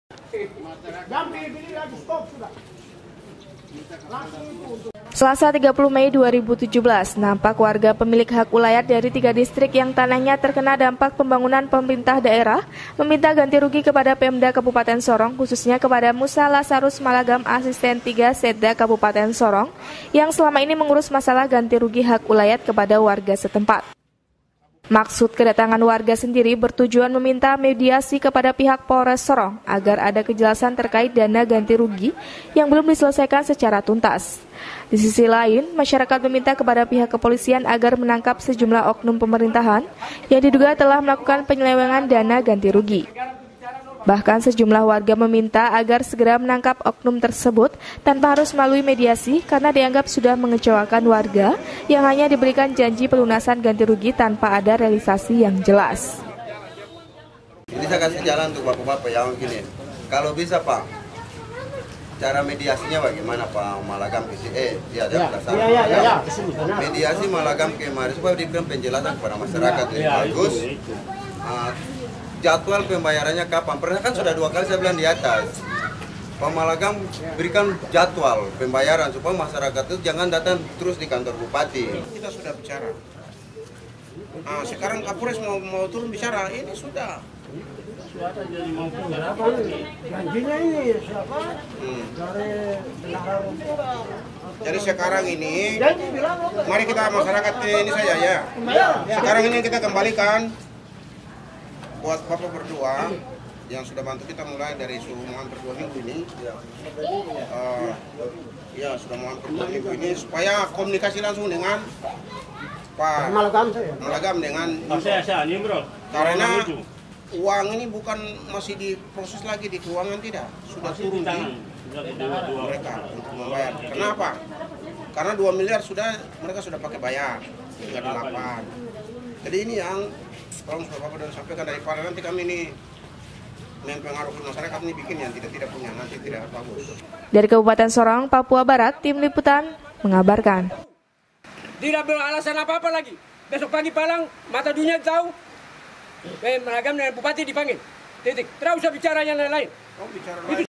MASYARAKAT 3 DISTRIK TUNTUT ASISTEN 3 BERTANGGUNG JAWAB TERKAIT GANTI RUGI
JANGAN LUPA SAKSIKAN KABAR SORONG PUKUL 20.00 WIT HANYA DI SORONG TV
BEKERJASAMA DENGAN BPJ TV CABLE (KABUPATEN SORONG) CP. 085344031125